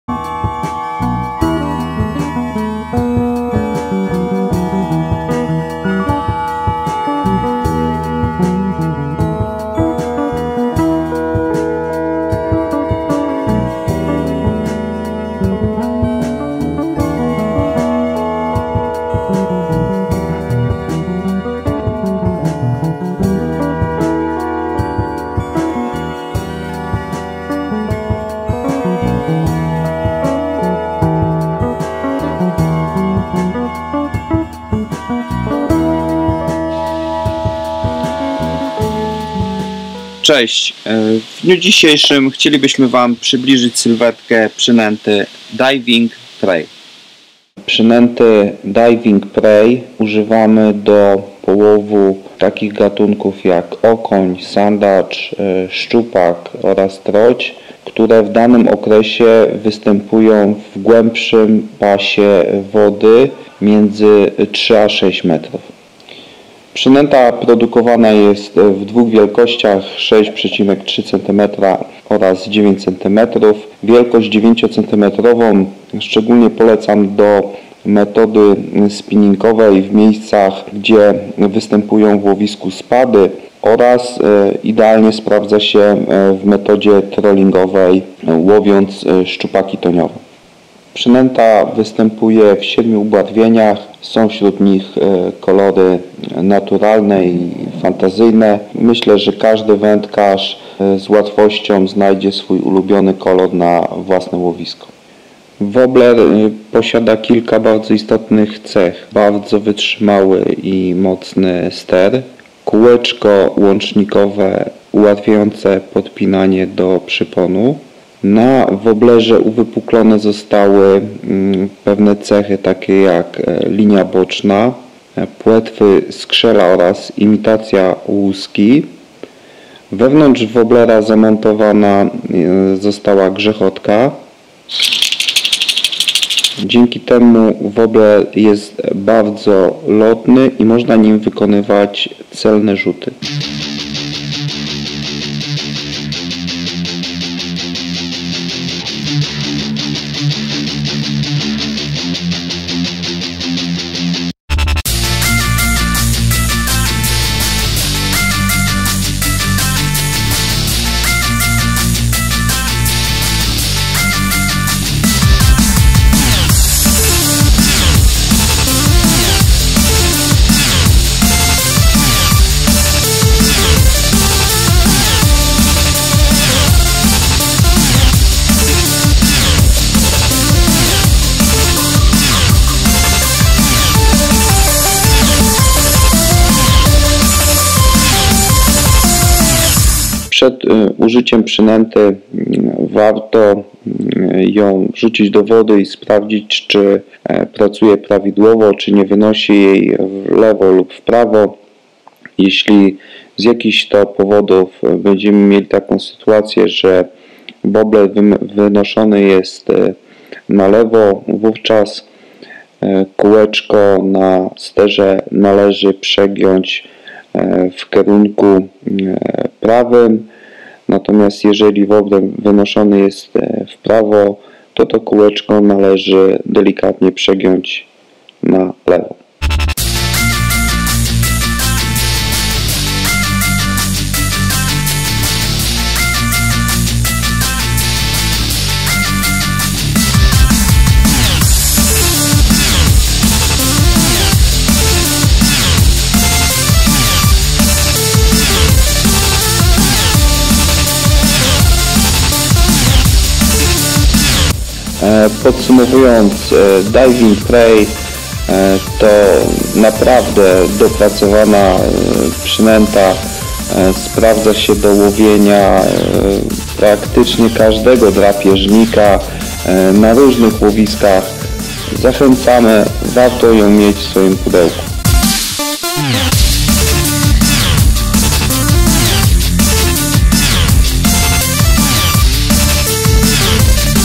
Savage Gear - Deep Diving Prey
Uniwersalna i diabelnie skuteczna seria woblerów na niemal każdy gatunek drapieżnych ryb.